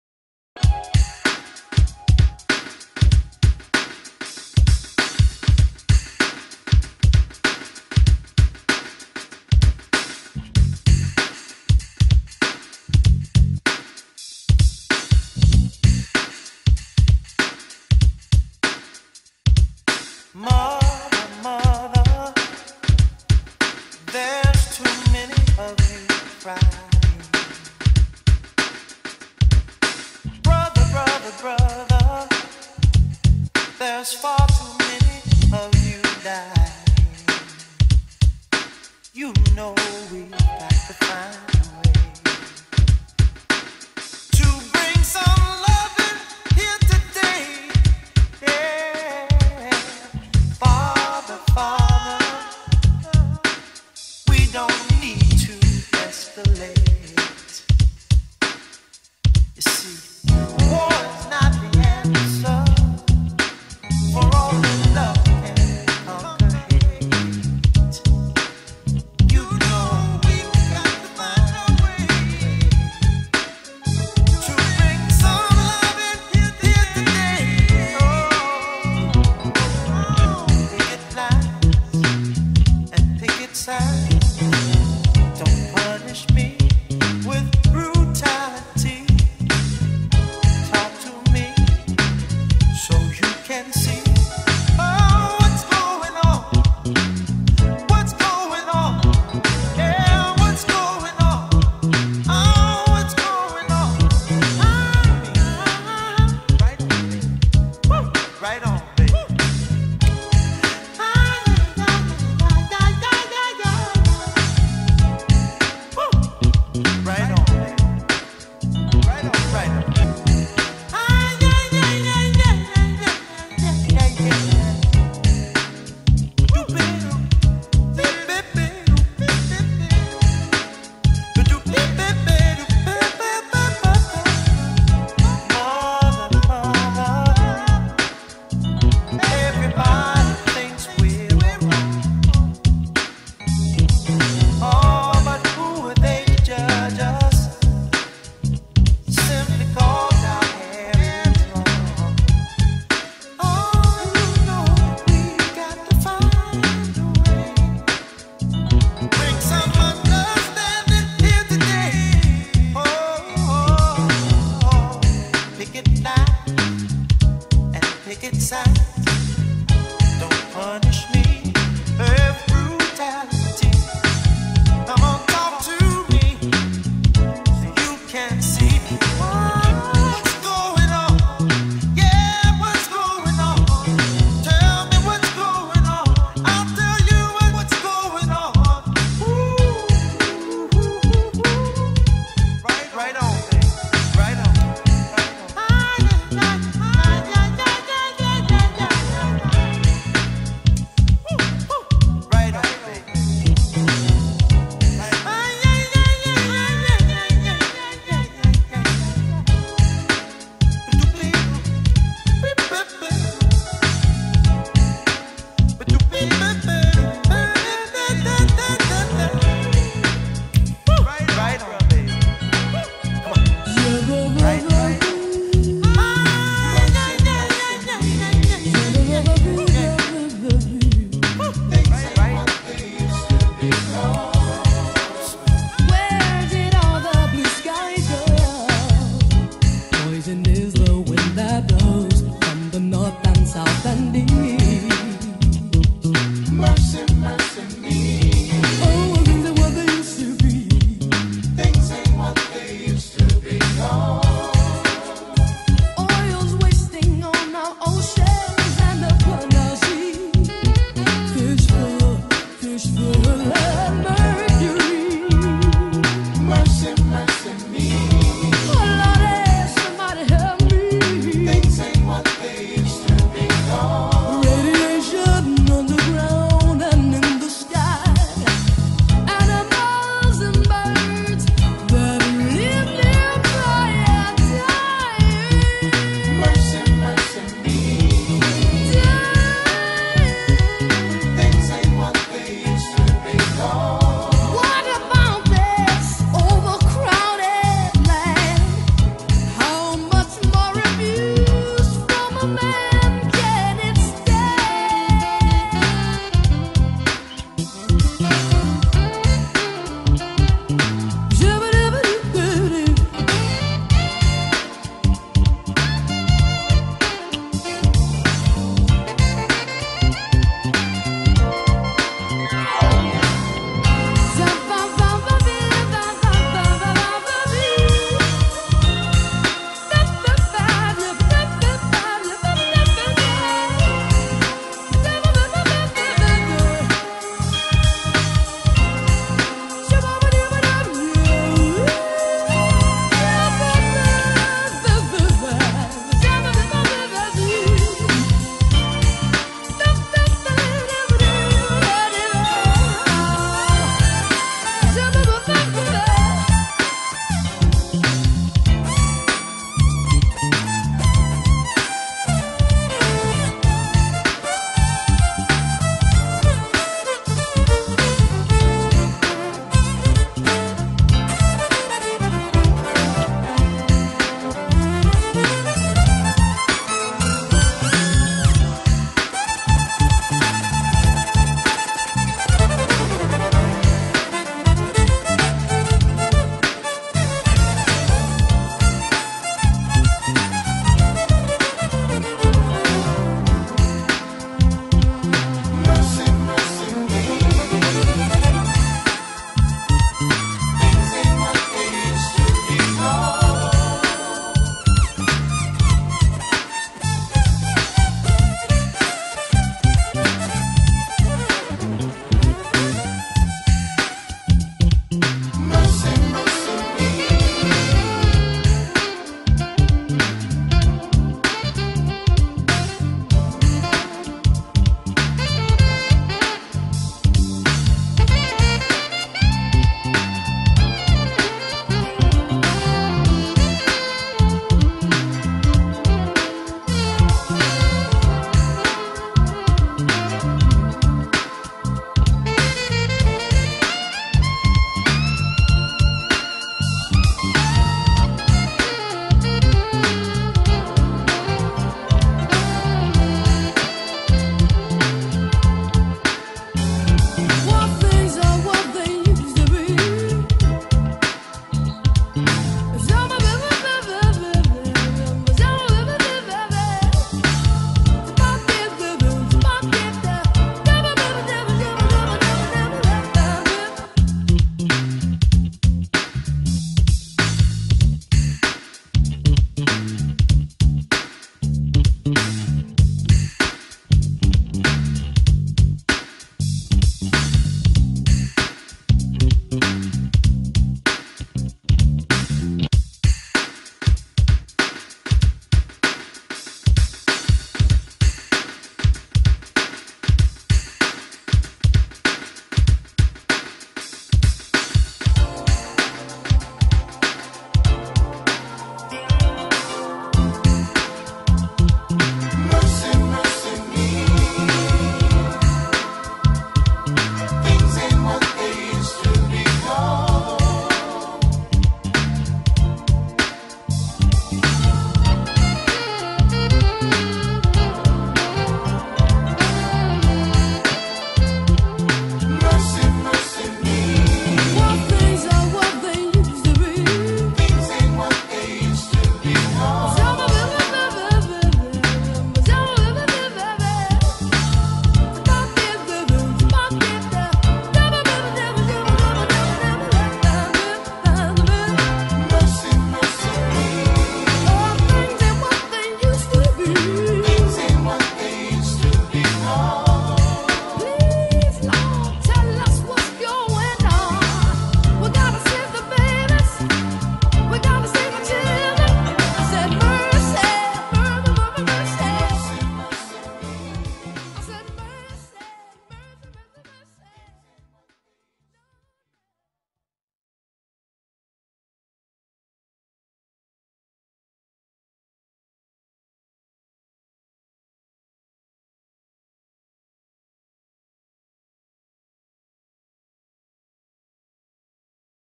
Marvin n Milira Remix
Two classic voices combined on this Tribute Remix to Marvin Gaye..Additional Vocals by Milira.